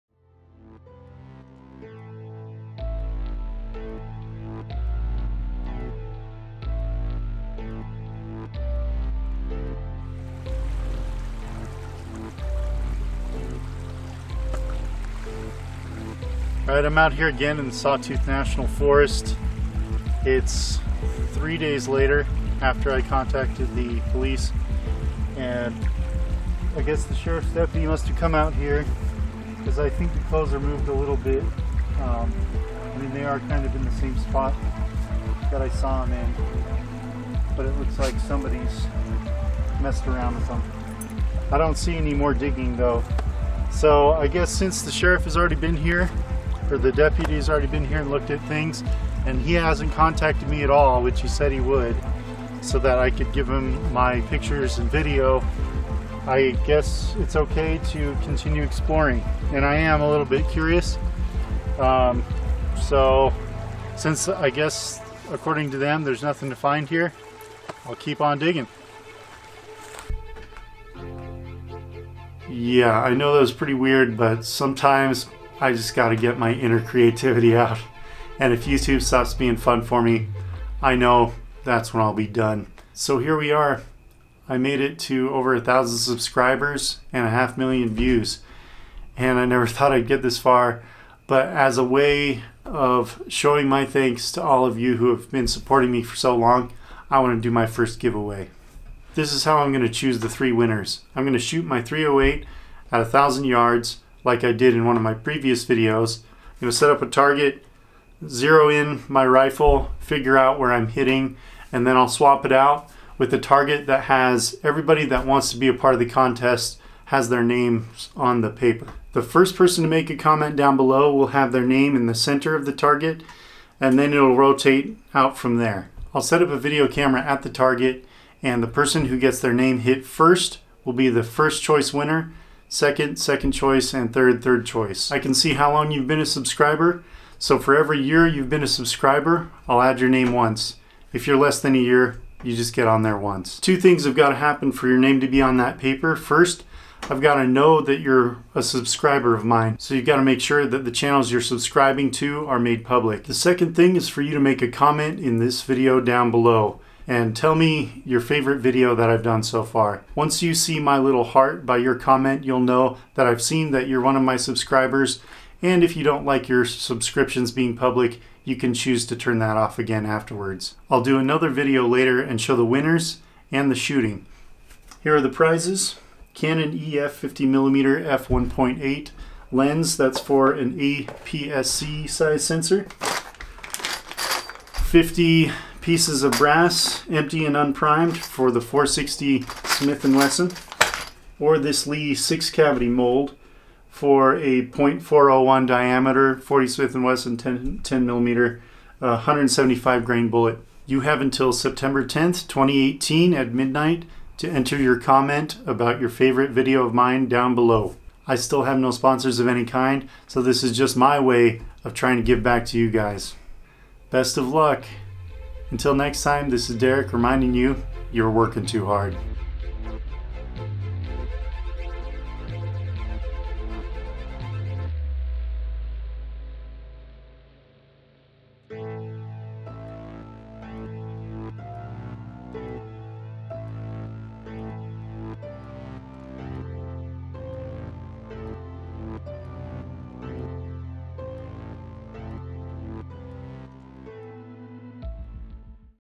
1,000 Subscriber Special and Prize Giveaway
I'm giving away prizes to 3 lucky winners. If you are subscribed and want to enter the contest for a prize, leave a comment below and tell me which one of my videos you like the best so far.